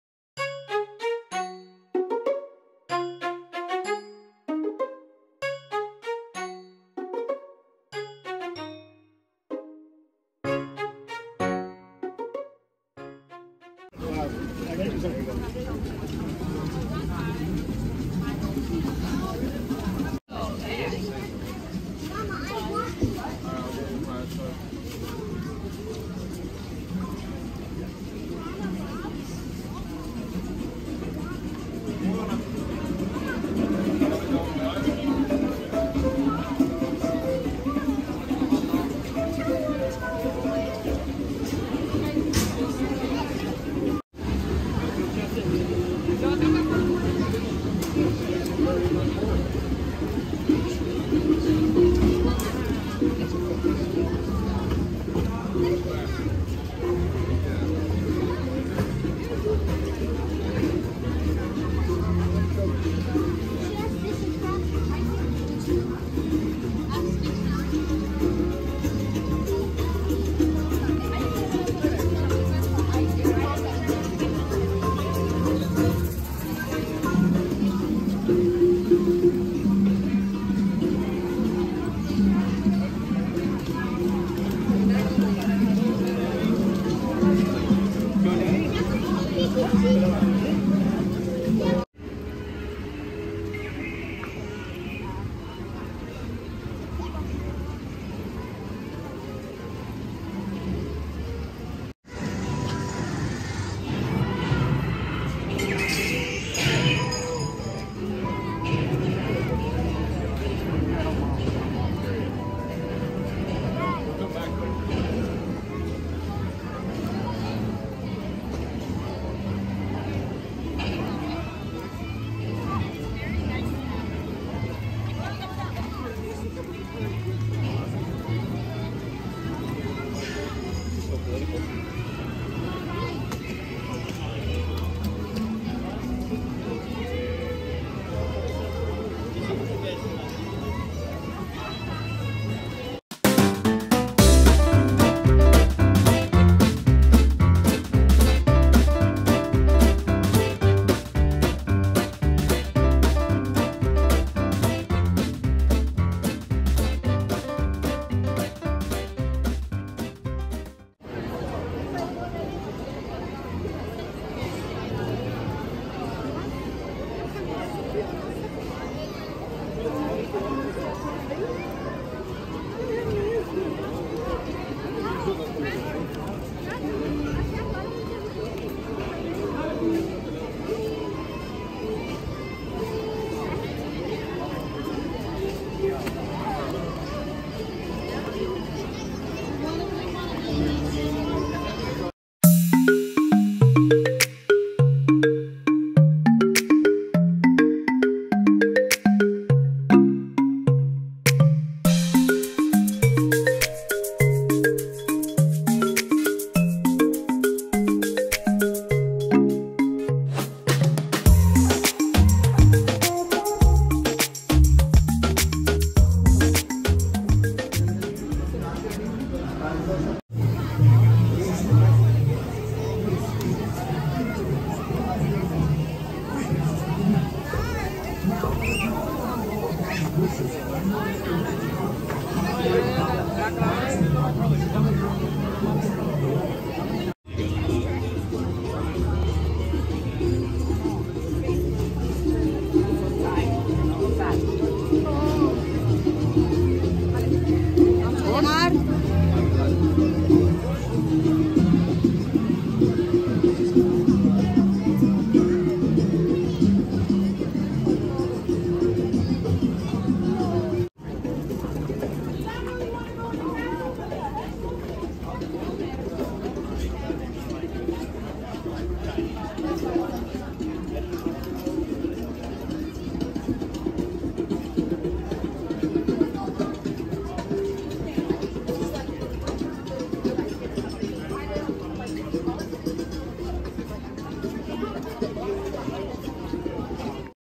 Why you WILL Love when it rains at Magic Kingdom!
In today’s video, I head to Disney World’s Magic Kingdom for a perfect, rainy evening! I stroll through Adventureland, and then Frontierland. Stop by the must see Country Bear Jamboree, as well as a ride Pirates of the Caribbean. It was a great evening!

Did you enjoy this video? I’d love to hear your comments.